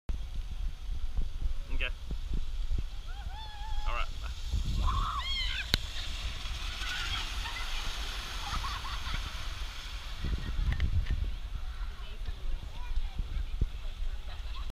water slide
Part of our trip to the water park with the kids from the Masonic Home of Ga